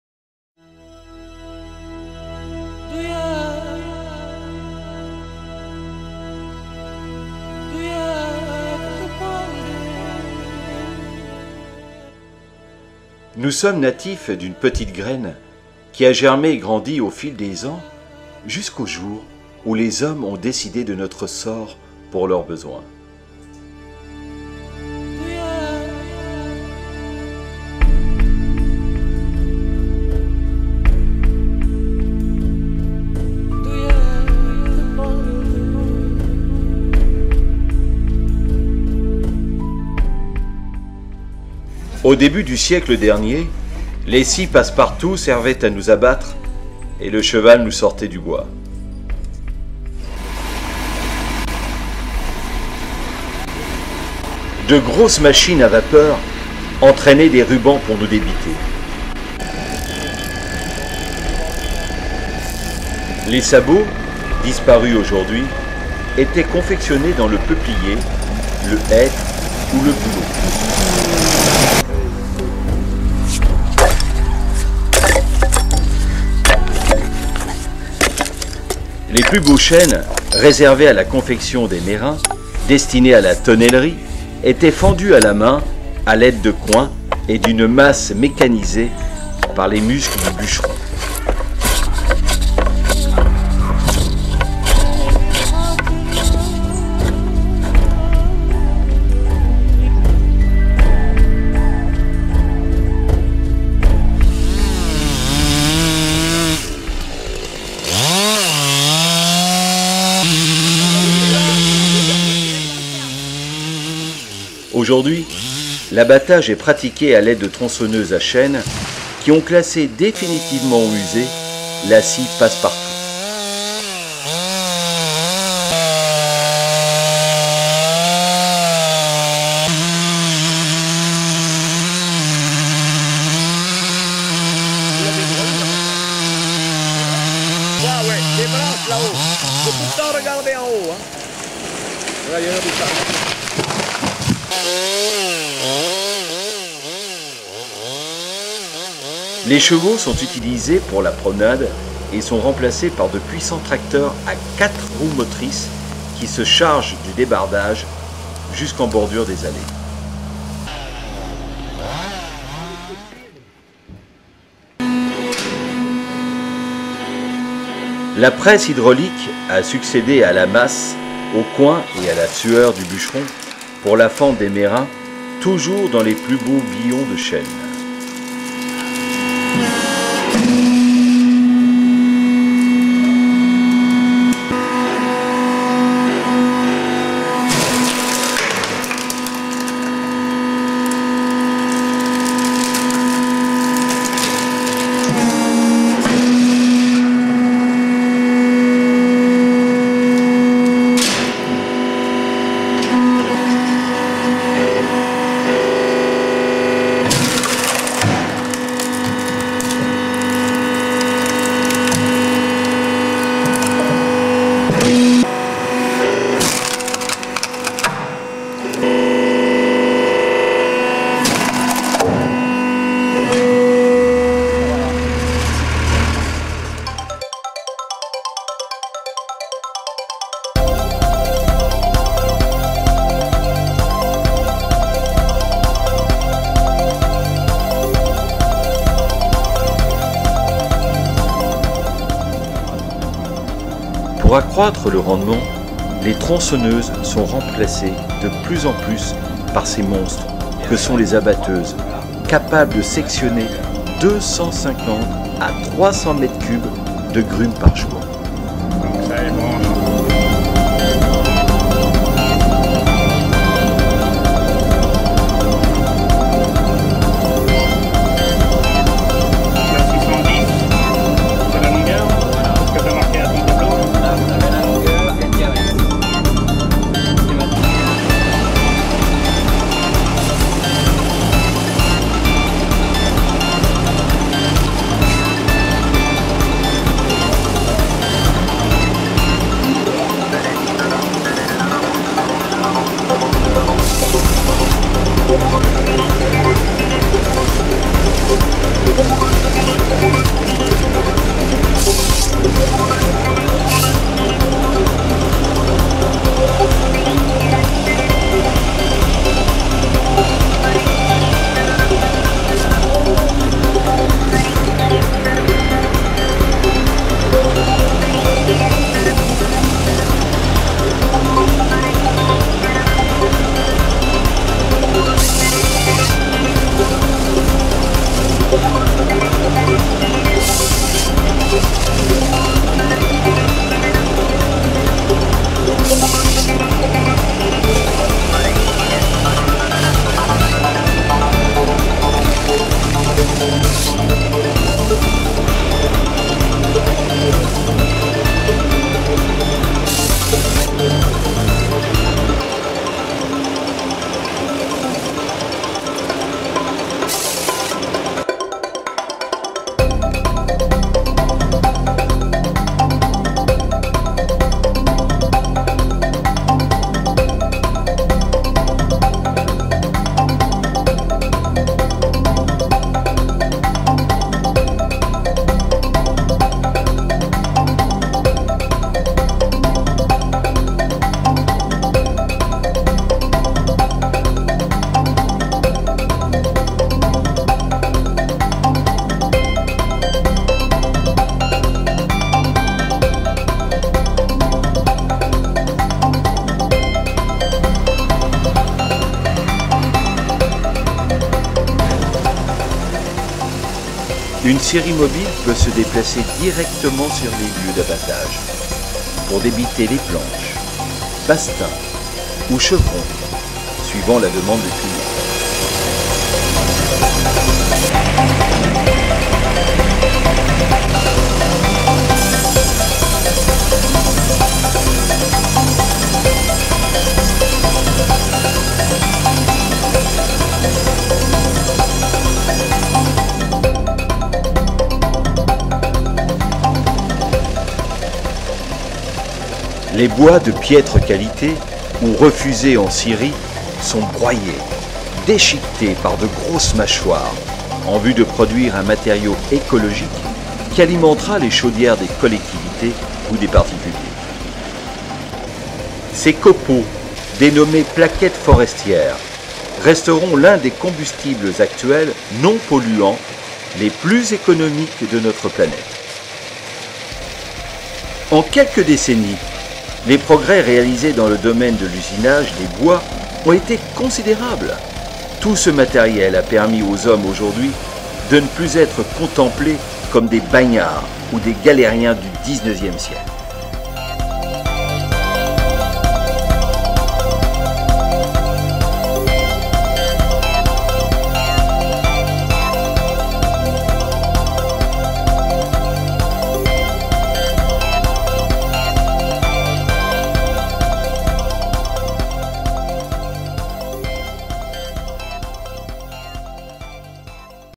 Les Bois JC divx
Travail du bois dans l'ancien temps par rapport à celui réalisé aujourd'hui dans les domaines de l'abattage, sciage, broyage...